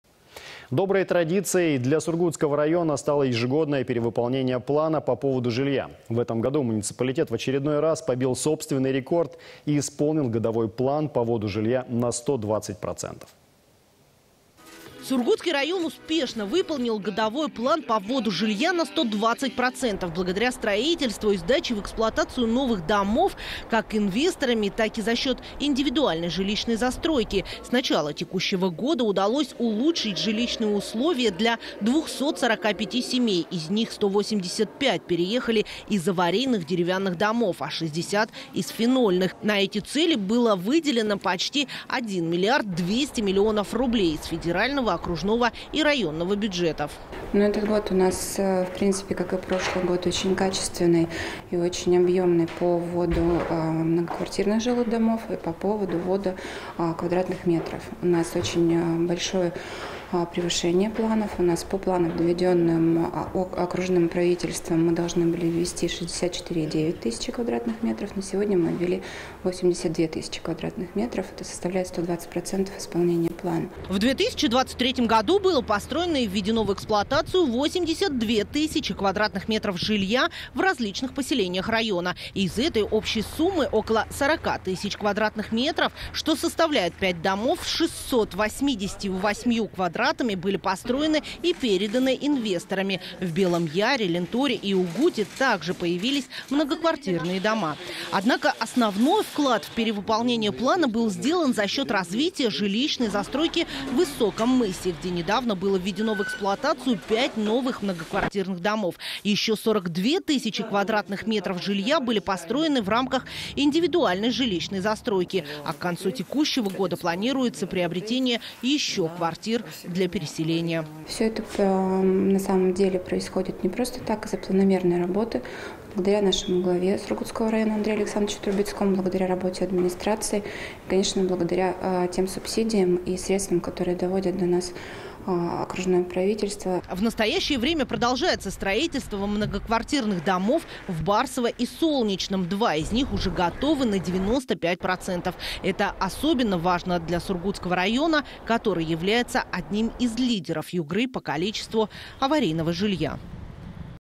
Сургутский район выполнил годовой план по вводу жилья на 120%
Доброй традицией для Сургутского района стало ежегодное перевыполнение плана по вводу жилья. Сургутский район успешно выполнил годовой план по вводу жилья на 120%, благодаря строительству и сдаче в эксплуатацию новых домов, как инвесторами, так и за счет индивидуальной жилищной застройки.